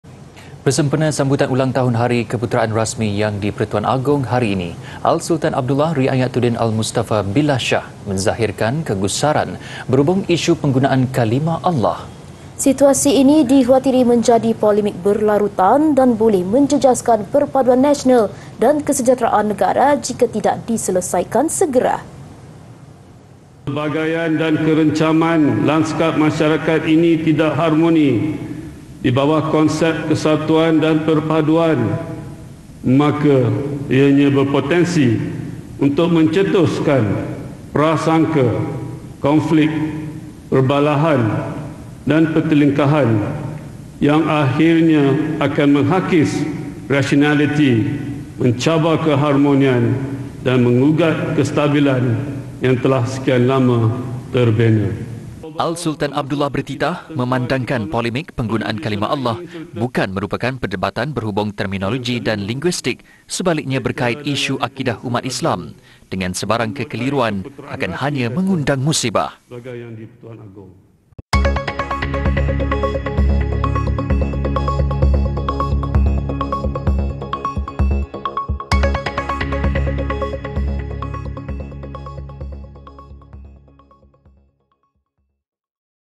Agong Gusar Isu Kalimah Allah Jadi Polemik Berlarutan
Bersempena sambutan Ulang Tahun Hari Keputeraan Rasmi Yang di-Pertuan Agong hari ini, Al-Sultan Abdullah Ri’ayatuddin Al-Mustafa Billah Shah menzahirkan kegusaran berhubung isu penggunaan kalimah “Allah”.